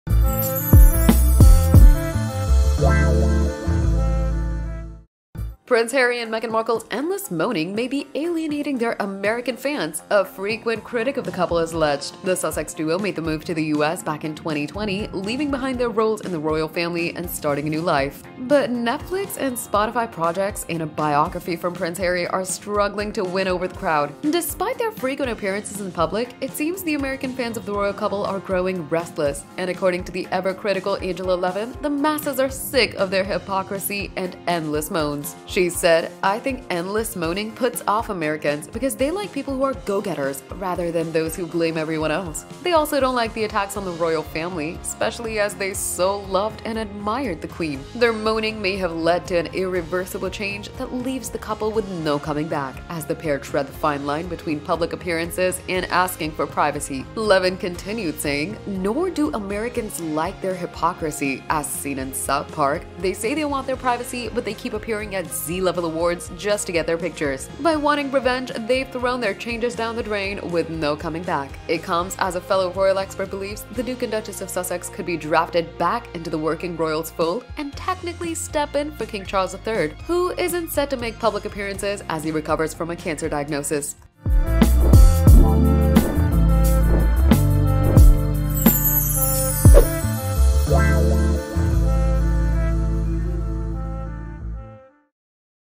Prince Harry, Meghan Markle getting on nerves of Americans
Americans are sick of prince Harry and Meghan Markle's hypocrisy and endless moanings about the Royal family.
Despite the couples' frequent appearances in public, they are losing face and image.